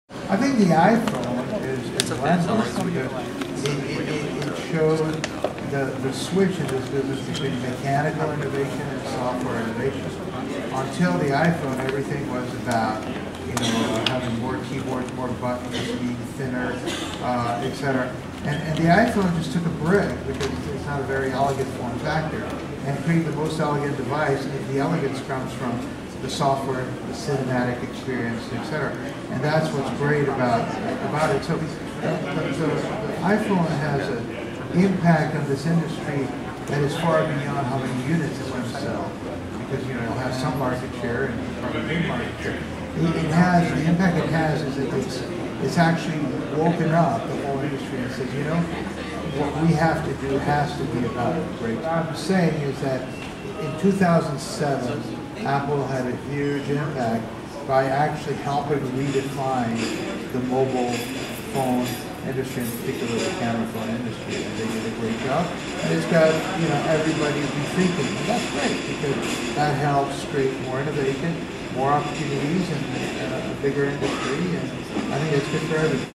Philippe Kahn Goes Full Power with Sensors/MEMS, iPhone nano
At the 6Sight Imaging conference in Monterey today, French legend Philippe Kahn unveiled his latest start-up after three years in stealth mode. Fullpower created a software that turns raw information gathered from sensors such as motion, imaging, proximity, ambient light, pressure, compass, GPS, etc... into actionable information instantaneously.
Currently the Wii and the iPhone are the most famous consumer electronic devices that make use of such motion sensors based on Microelectromechanical systems (MEMS) technology.

    "But the Wii and iPhone are just the beginning. Sensors will soon enable all kinds of functions, such as shaking your cell phone to pick up a call. No buttons, no fingers, just simple, natural gestures".

Kahn (pictured) seemed totally obsessed by what could happen in year 2010. Maybe because that's when Fullpower gets acquired, a similar pattern to his previous 2 companies that were sold about 7 yrs after their inception. The Frenchman indeed predicted that there will be 10 billion MEMS chips in mobile phones by 2010 and 2 billion new camera phones in year 2010 alone! All these will definitely require a piece of software to manage all that "raw" data! Here in more details what Fullpower' system does:

    The Fullpower multitasking real-time operating environment is a compact AI-system that uses a combination of rule based, neural network, and mathematical modeling algorithms. It's been designed to minimize integration efforts for the manufacturer of small devices like camera-phones, pacemakers, insulin pumps or media players and is available for Linux, BREW, Symbian, and Windows camera phones as well as other native environments.

Kahn is definitely onto something and again might have out speed any of its competitors again. French market research firm Yole Développement expects the global MEMS market to grow to $20 billion by 2016, from about $7 billion in 2006. This year, Yole expects about 400 million MEMS chip units to be shipped, or about 5 percent of the total foundry market.

Predicting the iPhone nano
Kahn was also totally gaga about the iPhone and even predicted the coming launch of a smaller version of the iPhone, that he called the "nano". The Fullpower CEO also said his company was working with most of the CE manufacturing using sensor which I assumes include also Apple!

The brick/iPhone vs the fashion model/Razr: software vs hardware innovation!
During his 6Sight keynote liken the iPhone to the "brick" and the Motorola Razr to a "fashion model".

    "The Razr is very thin, anorexic and fits in your shirt pocket. But it is not great from a camera phone stand point, because by shrinking it and making everything smaller, you can do less and less things with it. But it is a really cool device... [until the "brick" arrived!] The brick is an iPhone. The iPhone is big, its heavy, doesn't really have a great battery life. It doesn't fit into your shirt pocket. [...] The fashion model is an example of mechanical innovation, the brick is an example of software innovation. The Razr is a very complicated piece of hardware. It takes very special factories to make it, you have to etch these things, they have moving parts, it is extremely small with lots of complex stuff. The iPhone is a big piece of hardware with one big button, that is about it. And then, it is all software. But, it has the whole camera phone industry scrambling!"